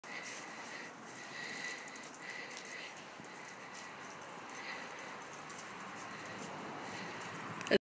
Andy_Feb2013
Andy is new to GGBR and available for adoption as of Feb 2013